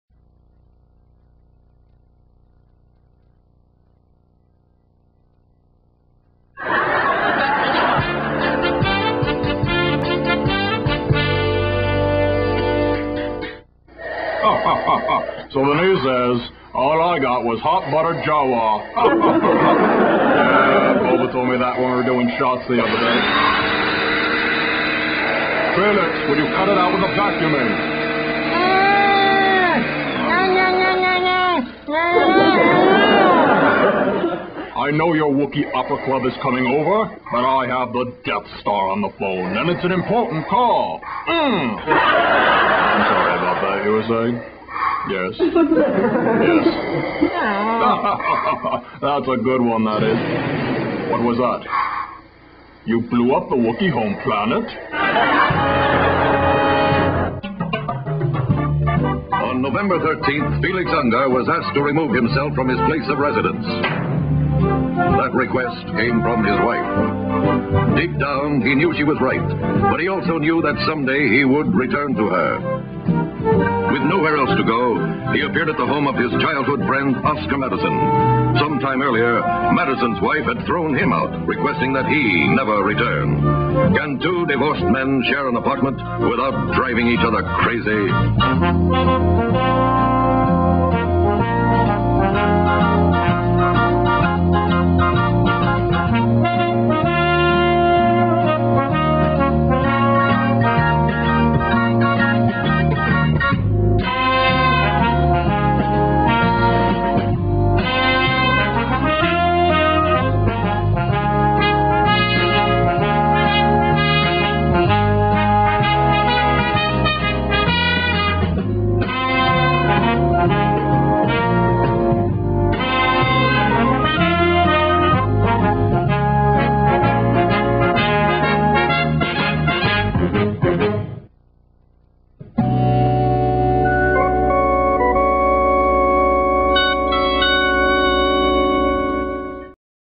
The Star Wars Odd Couple
Can two divorced intergalactic enemies share an apartment without driving each other crazy?